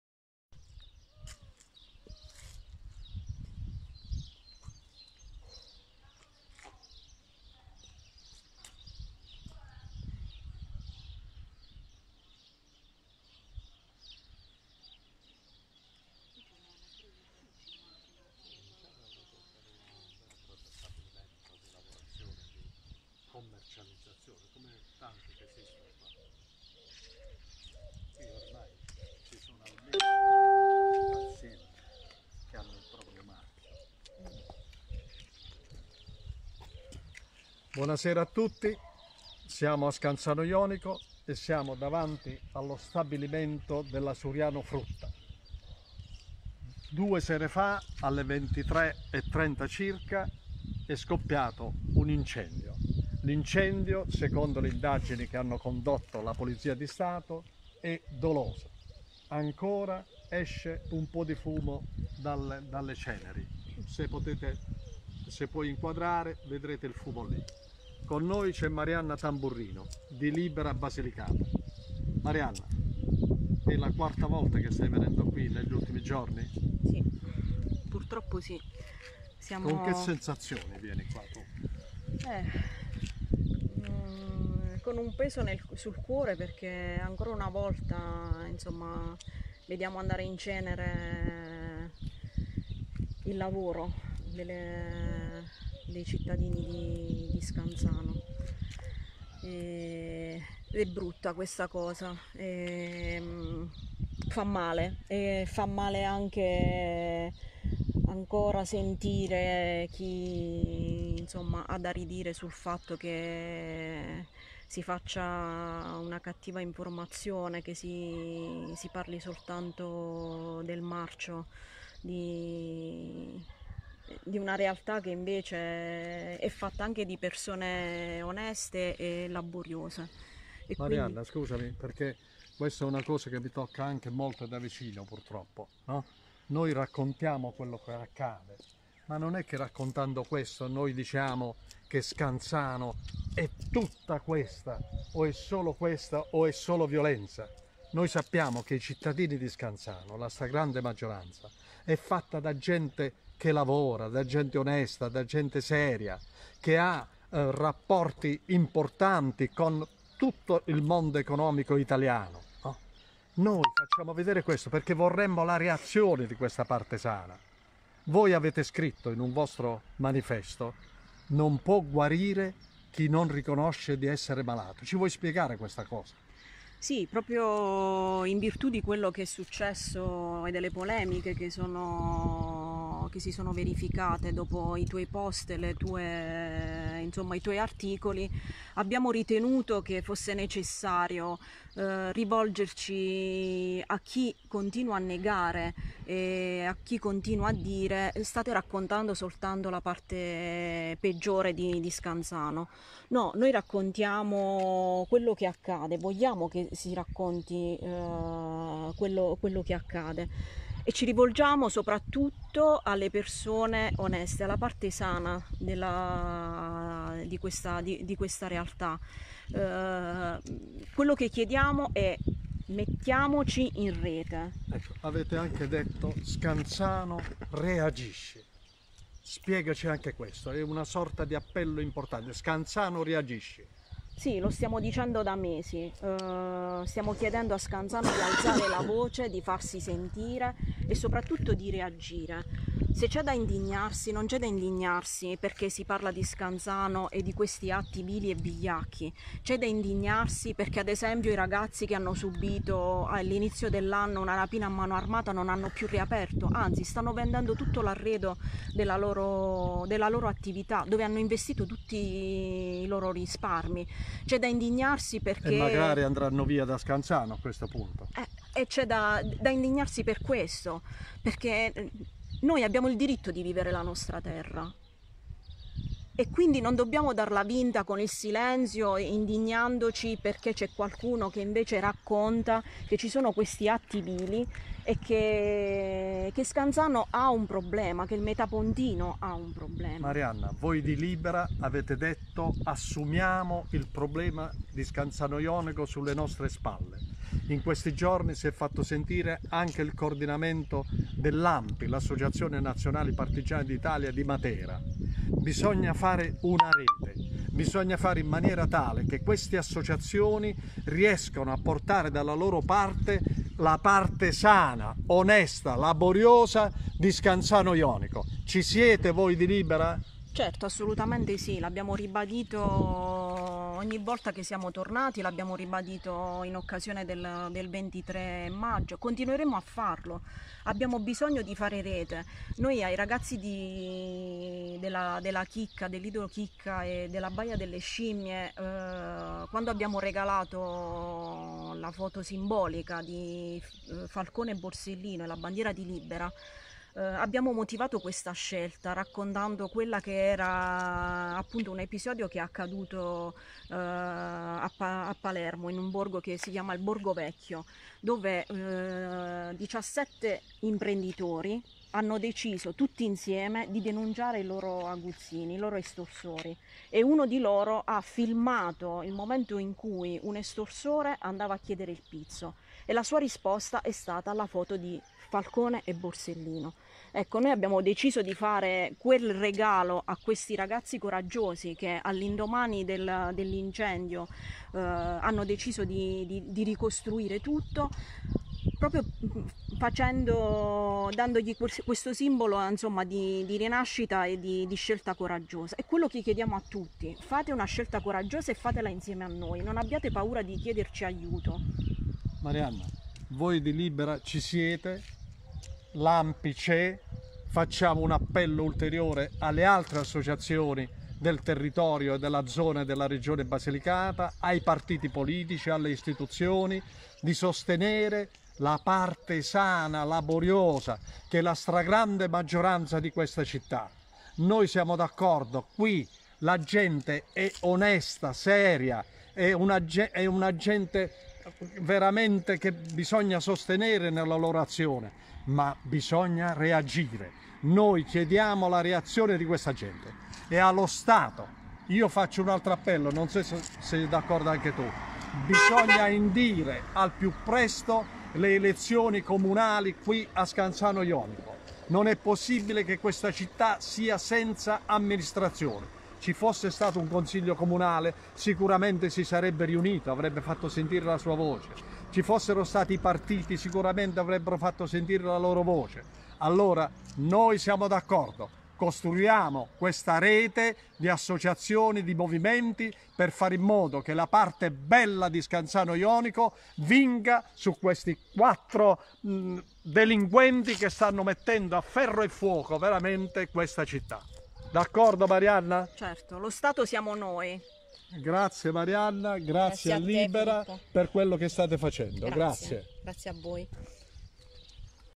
SCANZANO JONICO. LE INDAGINI SUI ROGHI CHE HANNO DISTRUTTO DUE LIDI E UN MAGAZZINO DO ORTODRUTTICOLI
RAFFORZATA LA PRESENZA DELLE FORZE DELL'ORDINE CON PATTUGLIE IN AZIONE ANCHE NELLE ORE NOTTURINE. INTANTO, SOPRALLUOGO DI LIBERA BASILICATA AL CAPANNONE DELLA SURIANO FRUTTA DEVASTATO DA UN INCENDIO MERCOLEDI' NOTTE. MARIANNA TAMBURRINO: "LA STRAGRANDE MAGGIORANZA DELLA GENTE DI SCANZANO JONICO, ONESTA, SANA, LABORIOSA, DEVE REAGIRE FACENDO RETE CON LE ASSOCIAZIONI CONTRO LA CRIMINALITA'. NOI CI SIAMO". E CHI DI DOVERE INDICA SUBITO LE ELEZIONI PER IL RINNOVO DEL CONSIGLIO COMUNALE